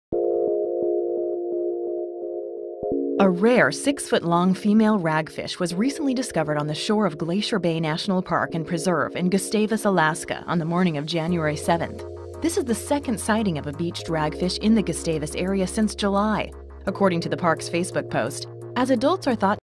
This Rare And Mysterious Sea Creature Washed Ashore In Alaska1
A rare, -six foot long female ragfish was recently discovered on the shore of Glacier Bay National Park and Preserve in  National Park